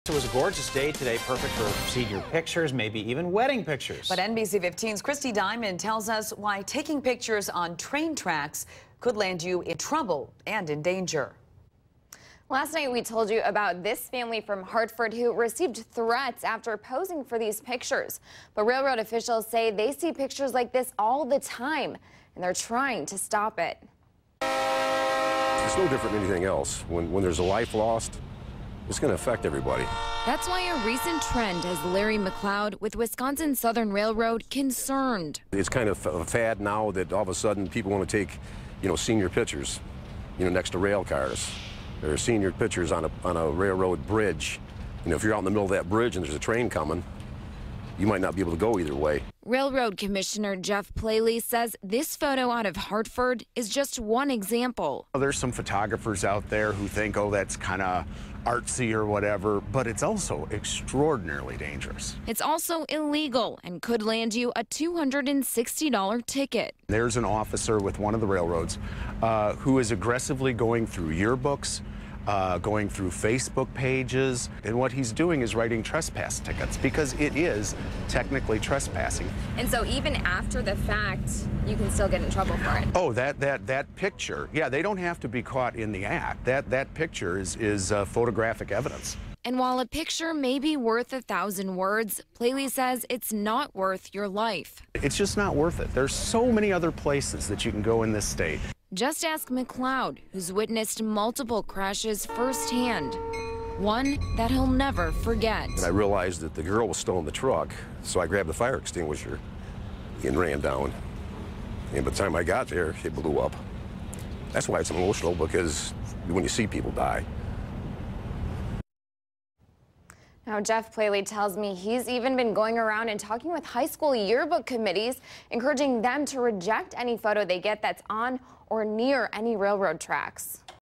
Dangers of Railroad Photos 10pm 06/17/2015
Officials are asking people to stop taking pictures on or near railroad tracks.